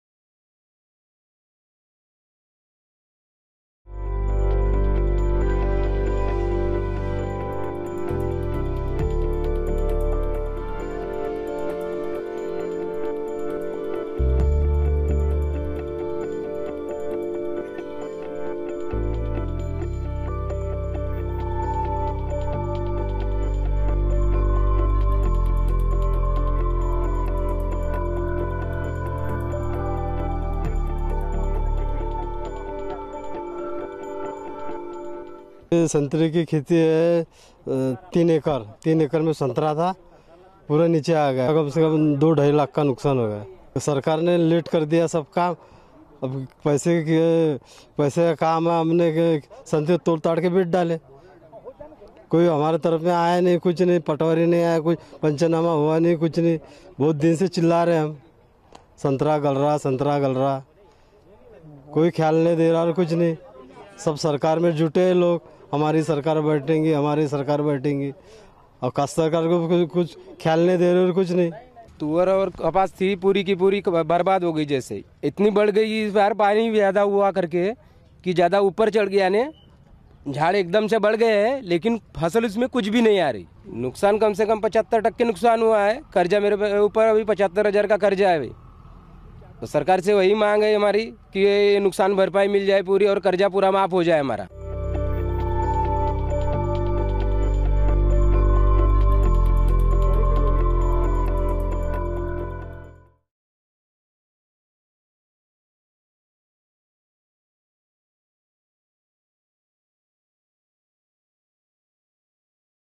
Sharad Pawar visits rain affected agricultural areas in Nagpur
Nagpur (Maharashtra): Nationalist Congress Party (NCP) chief Sharad Pawar visited rain affected agricultural areas of Nagpur district on November 14. One of the farmers said that no official visited them to witness the situation. “People are fighting to form government but no one is giving any focus on their situation.